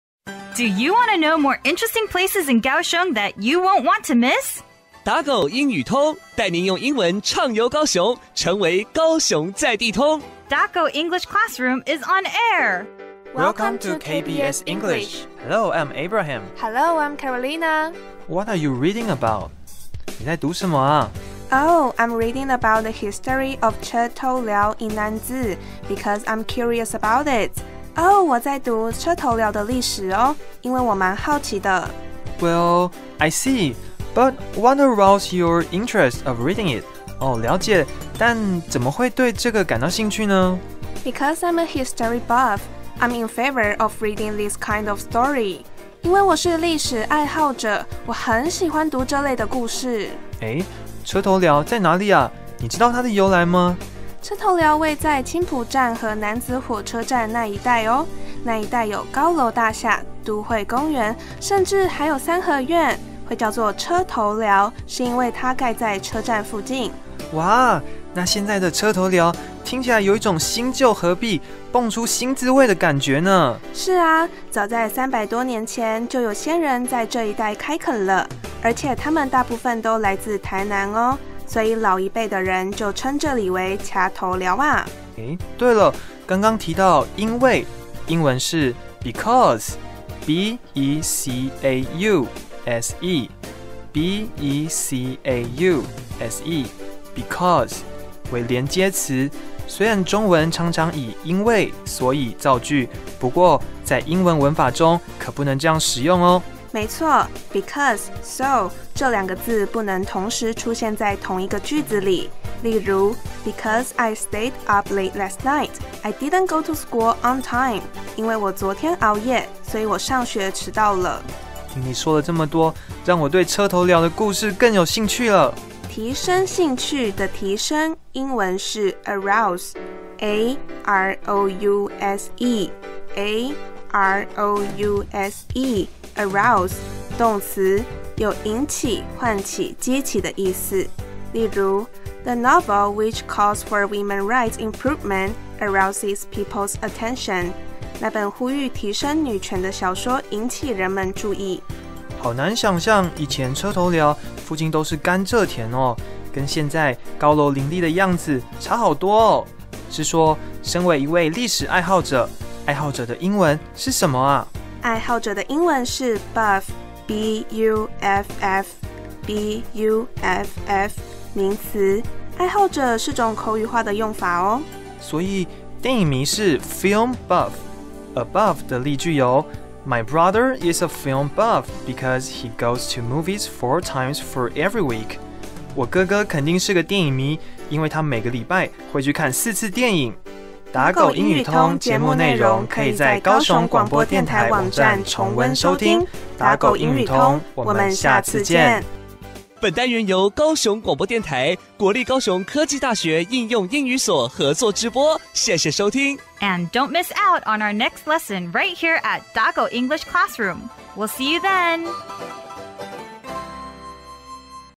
打狗英語通 1090324 Topic: 楠梓車頭寮 The history of Che-Tou-Liao in Nanzih
本日會話 Dialogue of the Day
1. What are you reading about?
你在讀甚麼啊？
2. Oh! I’m reading about the history of Che Tou Liao in Nanzih because I’m curious about it.
喔~我在讀車頭寮的歷史喔！因為我蠻好奇的。
3. Well…I see, but, what arouse your interest of reading it?
喔~了解。但…怎麼會突然對這個感到興趣呢？
4. Because I’m a history buff, I’m in favor of reading this kind of story.
因為我是歷史愛好者，我很喜歡讀這類的故事。
本日單字 Vocabulary of the Day
1. because [bɪˈkɑːz] (conj.) 因為，由於
2. arouse [əˈraʊz] (v.) 引起；喚起；激起
3. buff　　[drəʊn] (n.) …迷、愛好者